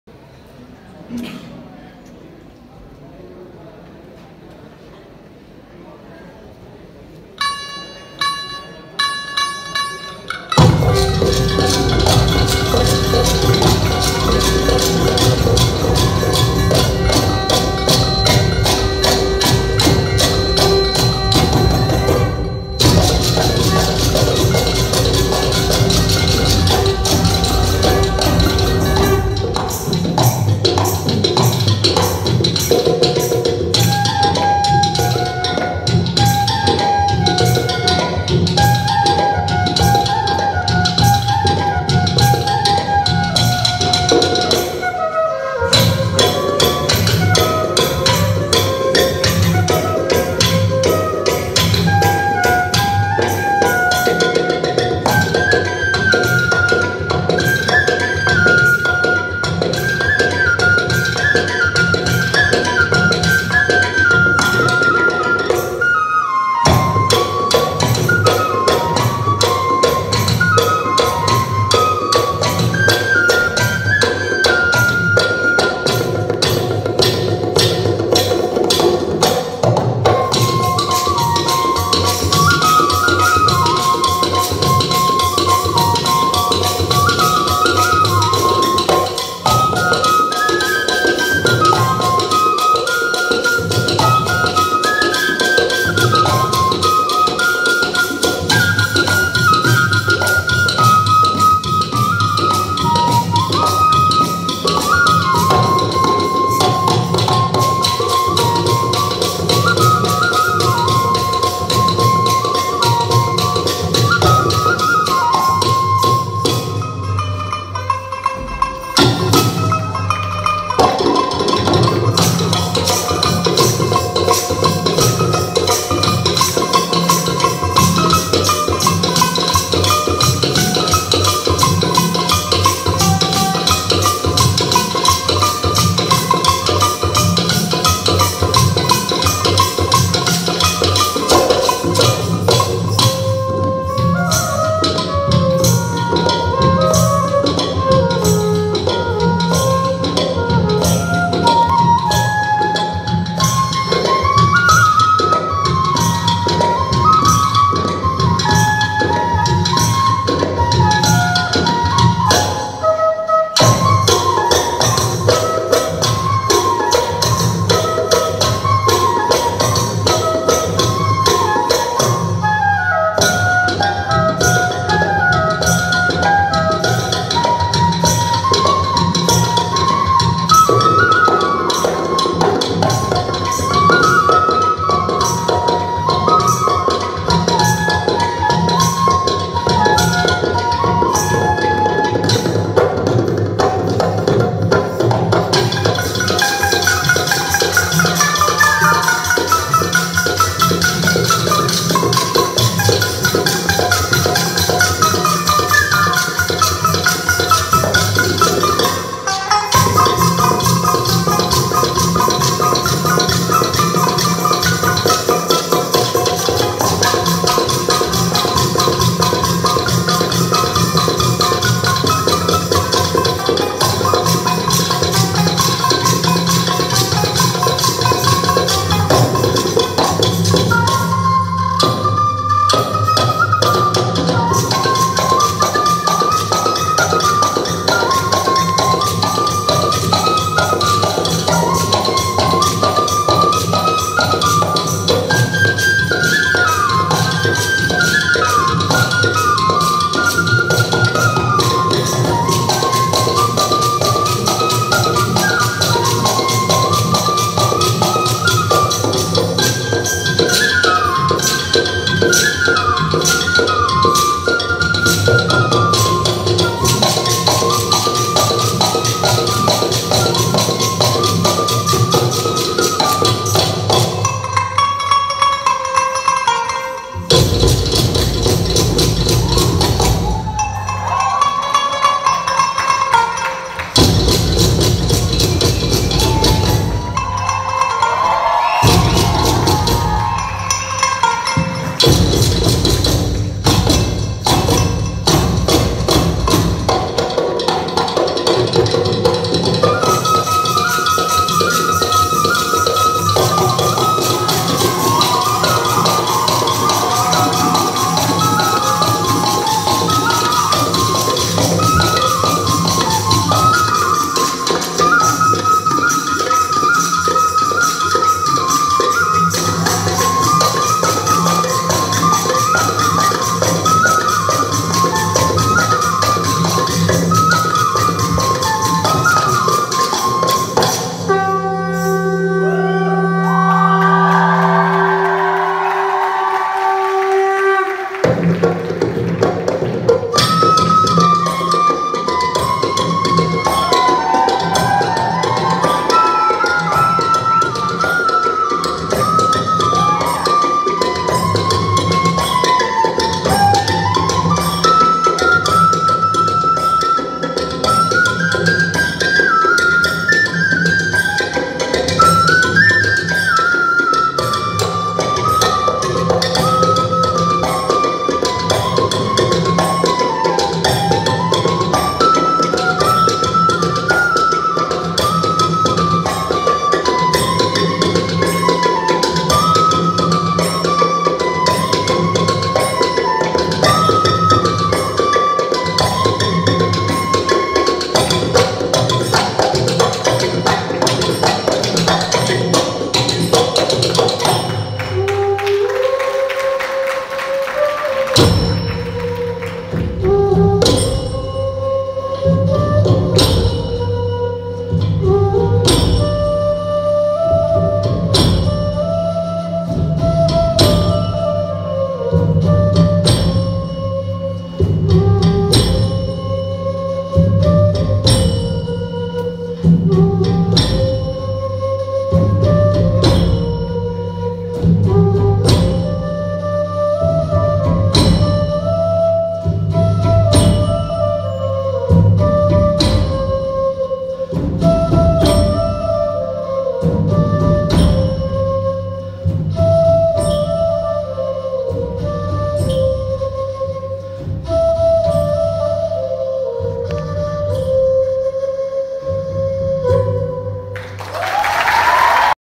Desh bhaghti songs play folk instrument Punjab puchd B.K.S college muhar's team
Baba kundan Singh college muhar team perform panjab university chandigarh
Jileawala bagh nu smarpit program